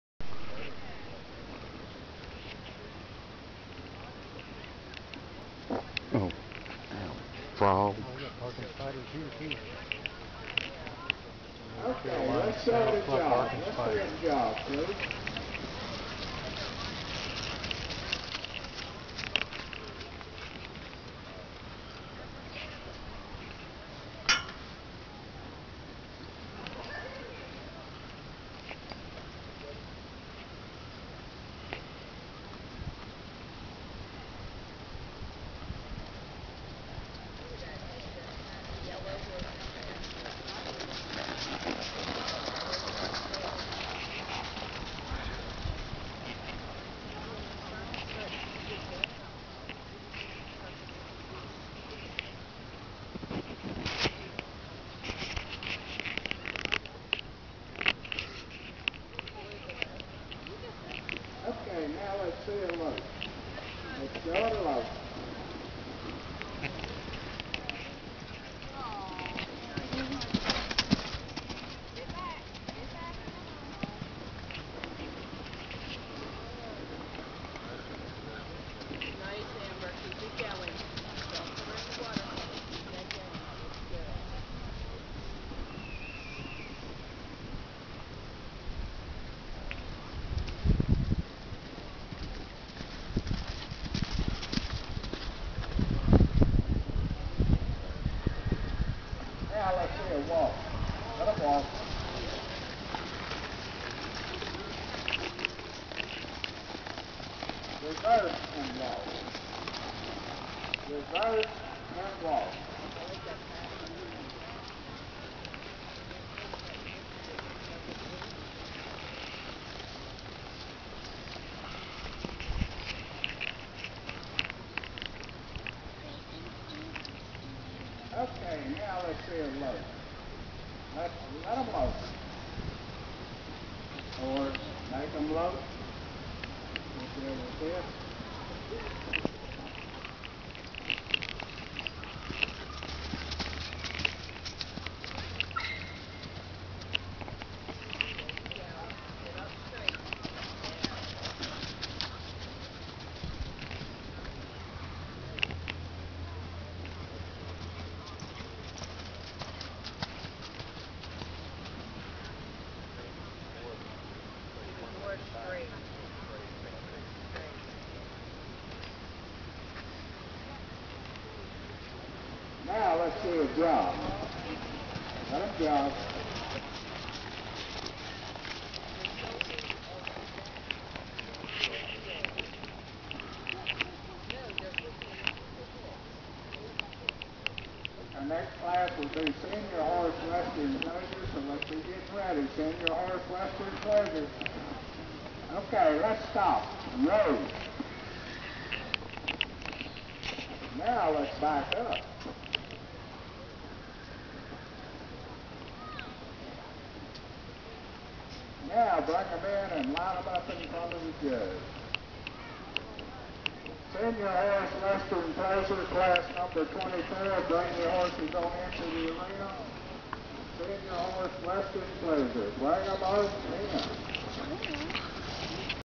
notice me first, apha mare for sale
this was at a local show , note this is her 3rd time being ridden in over a month so she did very well i had only ridden her twice to prepare her...